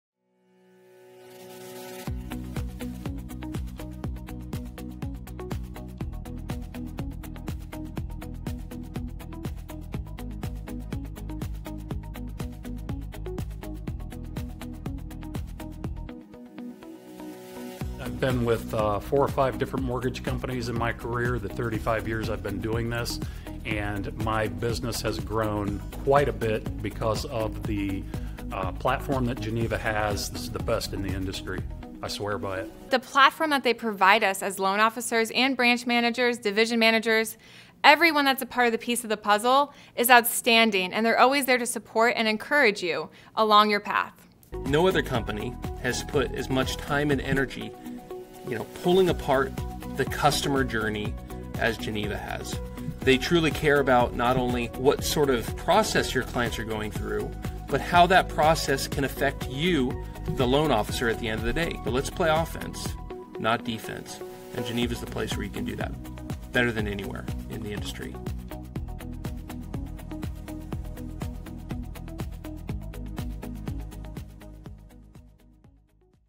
Mortgage Loan Officers & Branch Managers: Transform your career with Geneva Financial's innovati...
Mortgage Loan Officers & Branch Managers: Transform your career with Geneva Financial's innovative platform! 🌟

Experience the power of cutting-edge technology that streamlines your process so you can focus on what truly matters – building lasting relationships with your clients. 💼🤝

Visit JoinGFI.com to learn more and register for our next Discovery Day webinar!